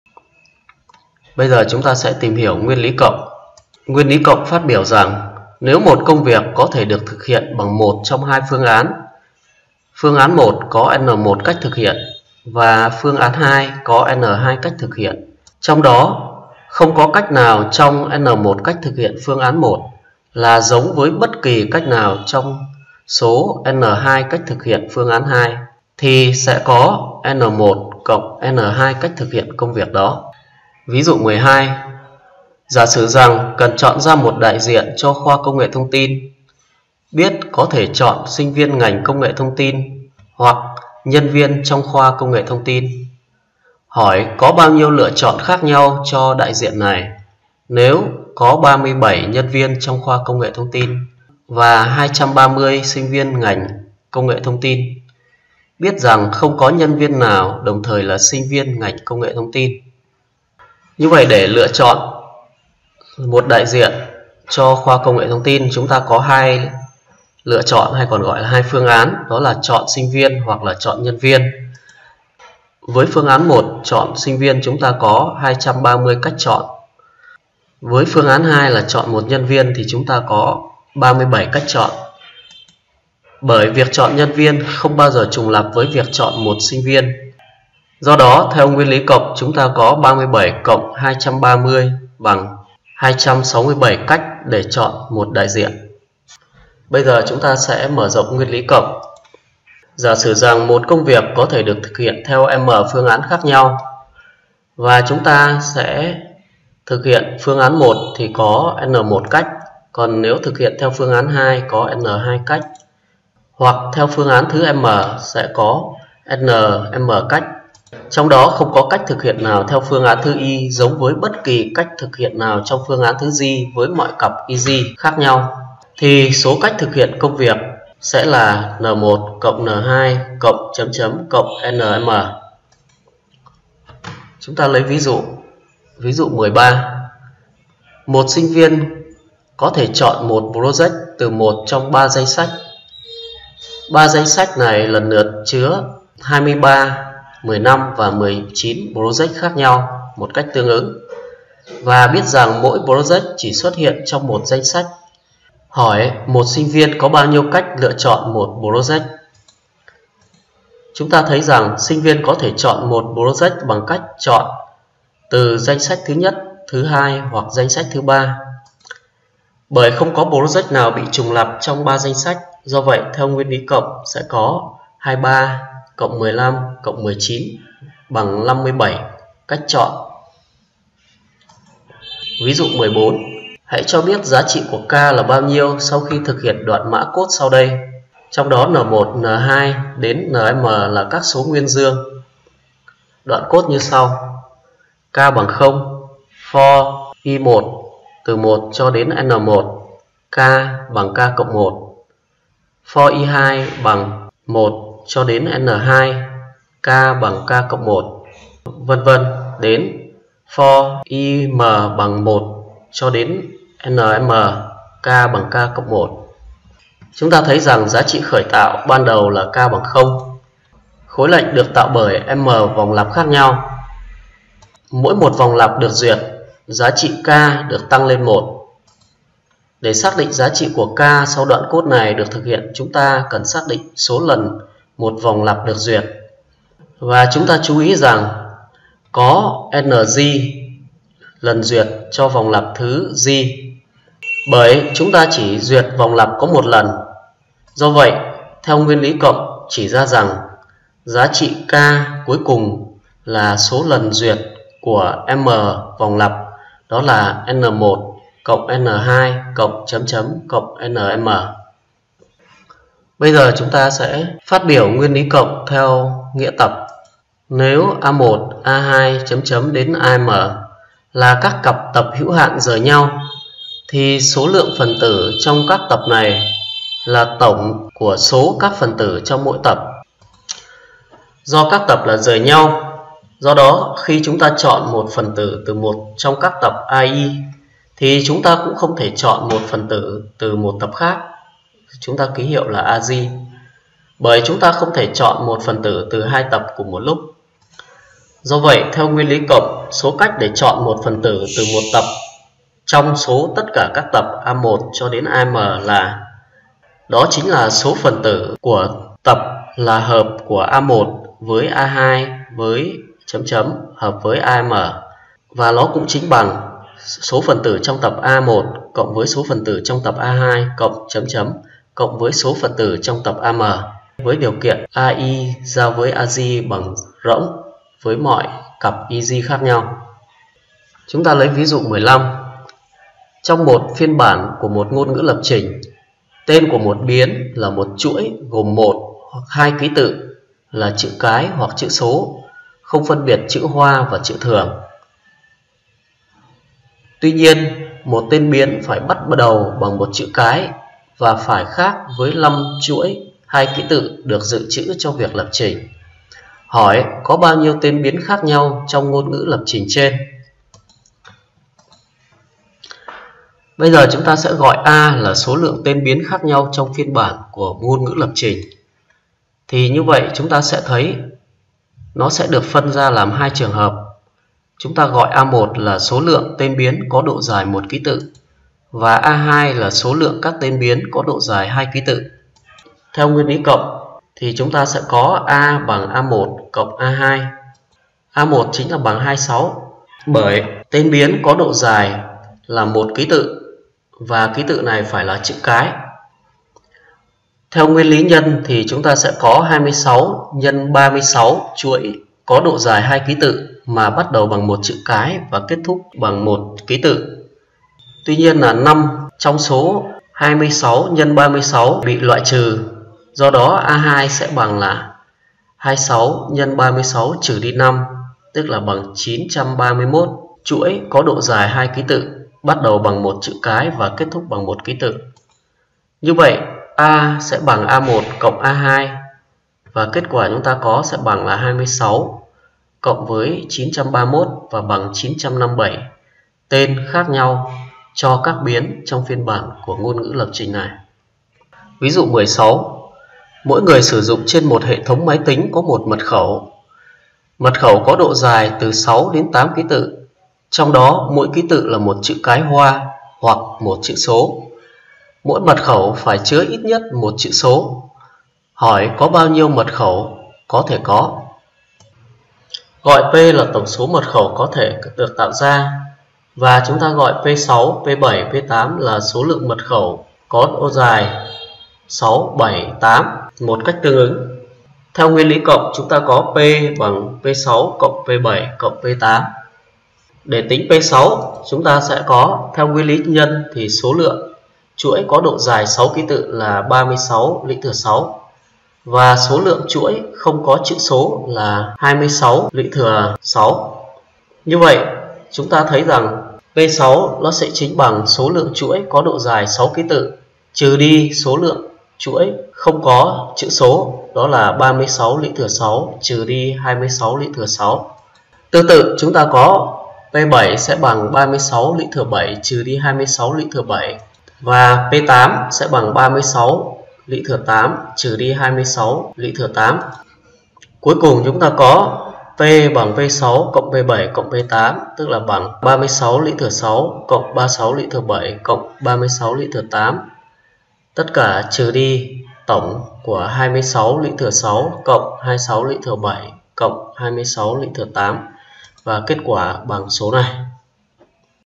#024 TOÁN RỜI RẠC Phương pháp đếm Nguyên lý cộng ducdvgtvt
Nội dung trình bày Các nguyên lý đếm cơ sở (Nguyên lý nhân, Nguyên lý cộng, Nguyên lý trừ, Nguyên lý chia), Nguyên lý Dirichle và lý thuyết Tổ hợp.

Tổ hợp, nghiên cứu sự sắp xếp các đối tượng, là một phần quan trọng của toán rời rạc. Chủ đề này được nghiên cứu từ thế kỷ 17, khi những câu hỏi tổ hợp nảy sinh khi nghiên cứu các trò chơi đánh bạc, cá độ.